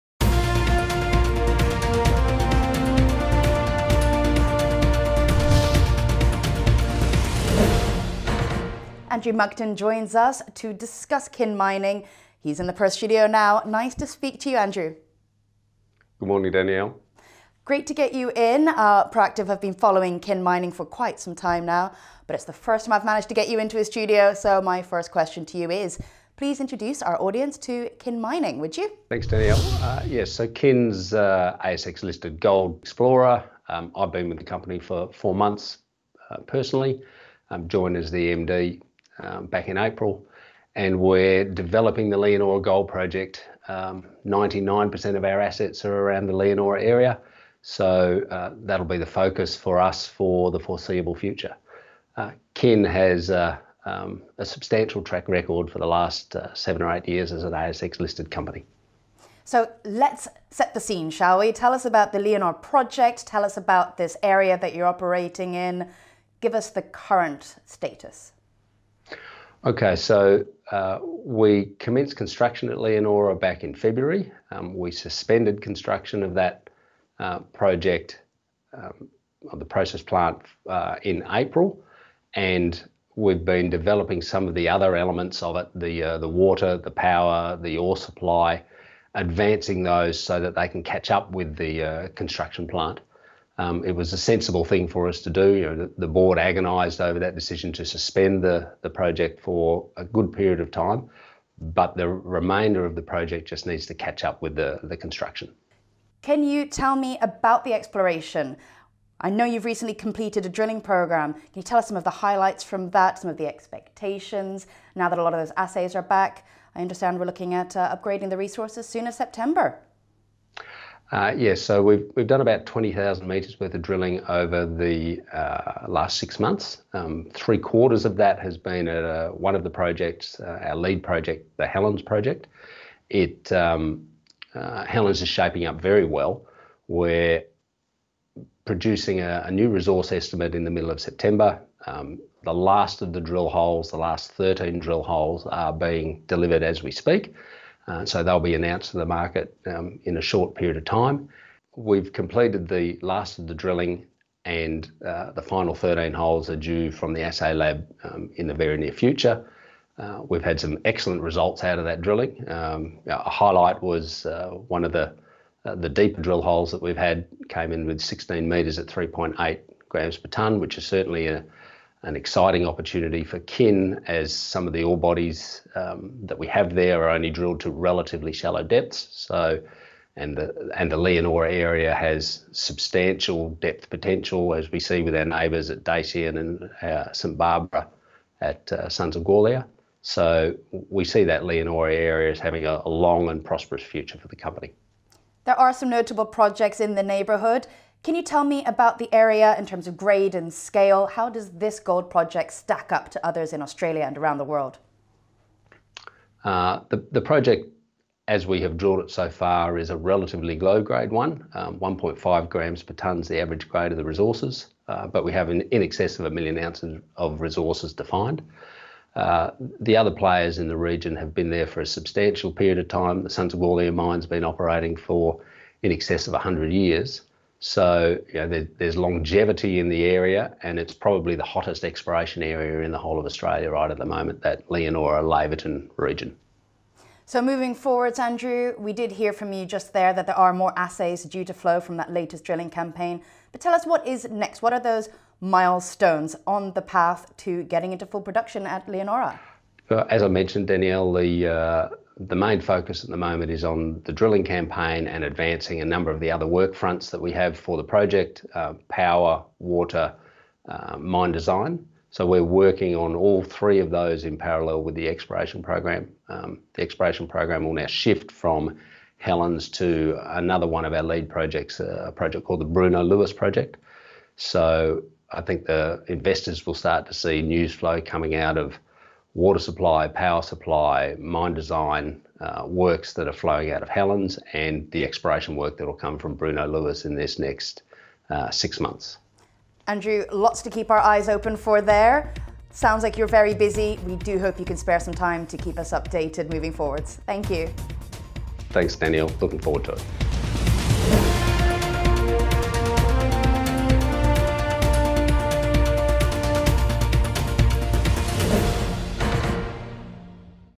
Kin Mining receiving assays, on track for updated resource estimate in September
Andrew Munckton, newly appointed managing director of Kin Mining NL (ASX:KIN), speaks to Proactive Investors about the emerging gold production company's wholly-owned Leonora Gold Project, located in the highly prospective North-Eastern Goldfields region of Western Australia.

Kin has suspended construction at the Cardinia process plant site, as it works to develop utilities and infrastructure in the area, and simultaneously grow its resources. An updated resource estimate is on track to be announced to the market in September.